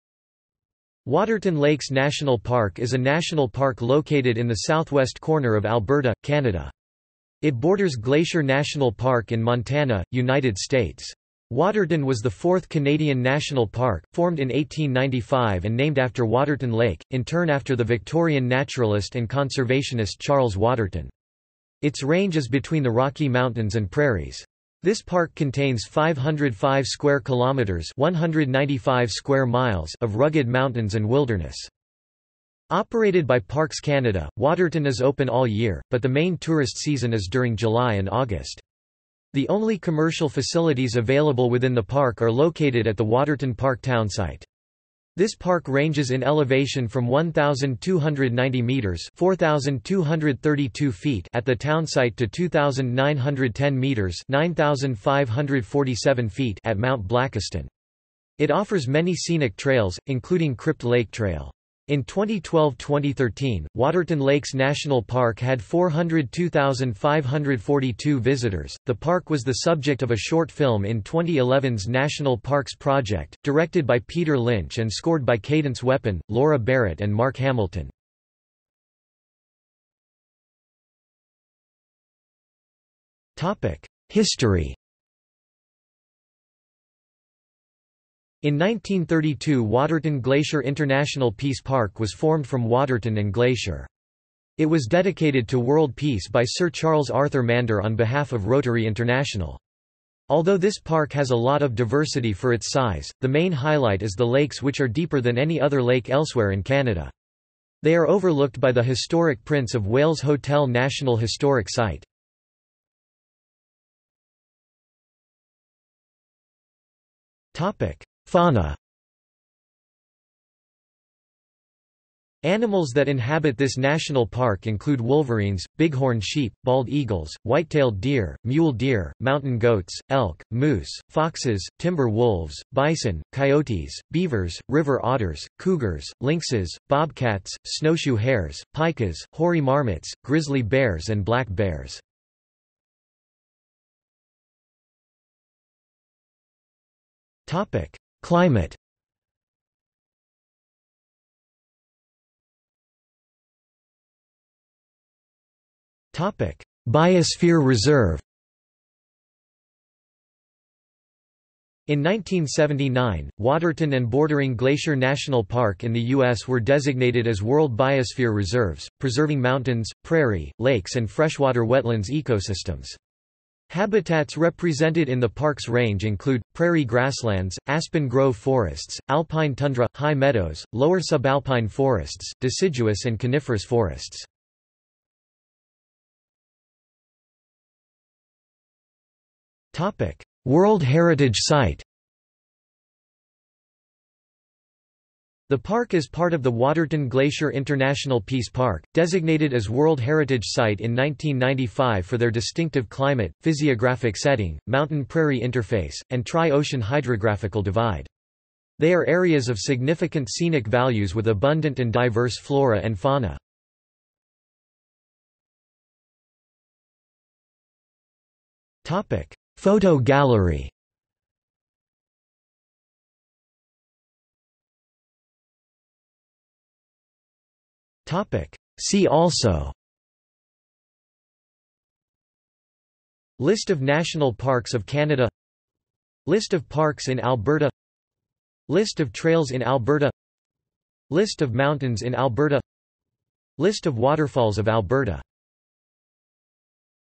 Waterton Lakes National Park | Wikipedia audio article
This is an audio version of the Wikipedia Article:
Waterton Lakes National Park

00:01:19 1 History
00:01:53 2 Fauna
00:02:23 3 Climate
00:02:32 4 Biosphere Reserve
00:03:06 5 World Heritage Site
00:03:35 6 Photo gallery
00:03:44 7 See also

- Socrates

SUMMARY
Waterton Lakes National Park is a national park located in the southwest corner of Alberta, Canada. It borders Glacier National Park in Montana, United States. Waterton was the fourth Canadian national park, formed in 1895 and named after Waterton Lake, in turn after the Victorian naturalist and conservationist Charles Waterton. Its range is between the Rocky Mountains and prairies. This park contains 505 km2 (195 sq mi) of rugged mountains and wilderness.
Operated by Parks Canada, Waterton is open all year, but the main tourist season is during July and August. The only commercial facilities available within the park are located at the Waterton Park townsite. This park ranges in elevation from 1,290 metres (4,232 ft) at the townsite to 2,910 m (9,547 ft) at Mount Blakiston. It offers many scenic trails, including Crypt Lake trail. In 2012/2013, Waterton Lakes National Park had 402,542 visitors.The park was the subject of a short film in 2011's National Parks Project, directed by Peter Lynch and scored by Cadence Weapon, Laura Barrett and Mark Hamilton.